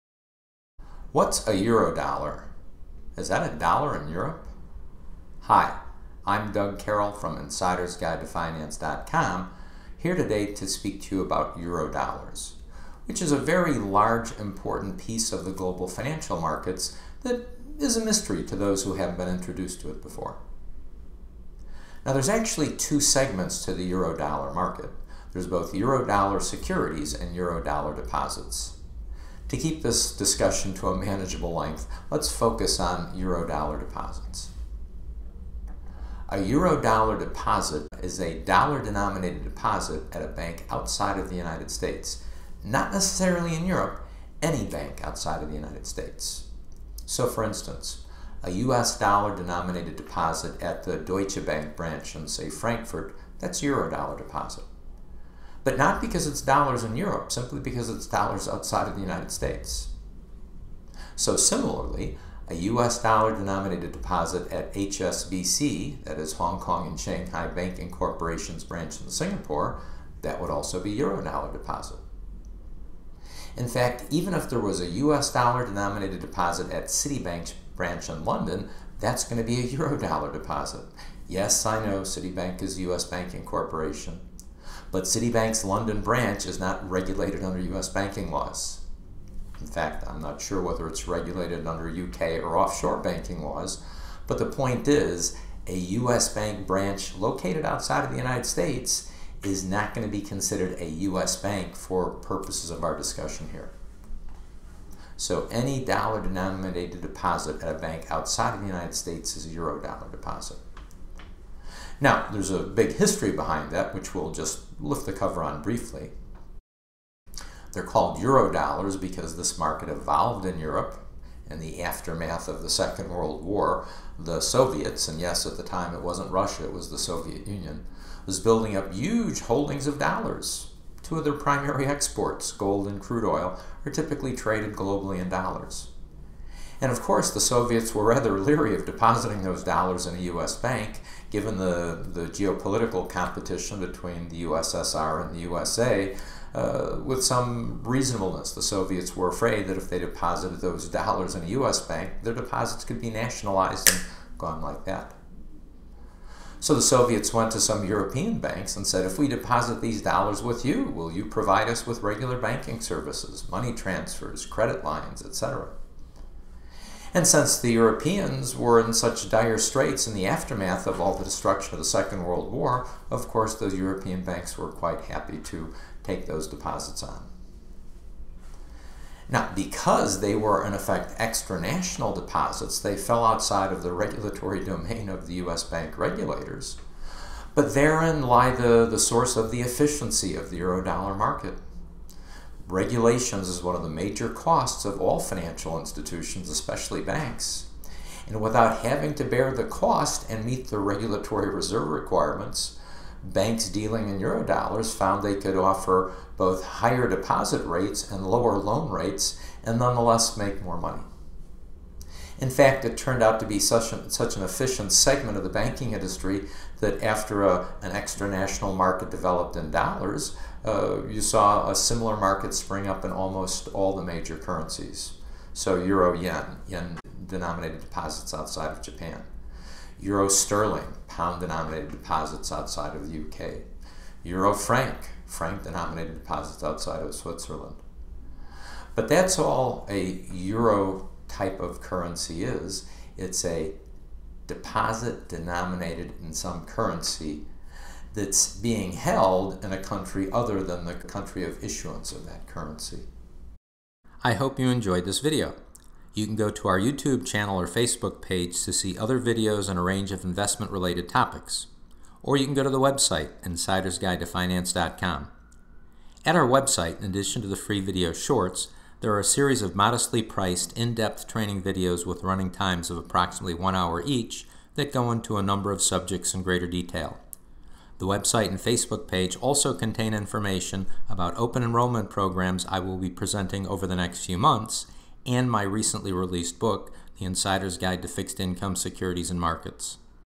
Eurodollars
This video introduces viewers to the basics of Eurodollar deposits. The presentation provides viewers with an explanation of what Eurodollars are, the factors that lead to the development of the Eurodollar market and the reasons that facilitated its growth over time.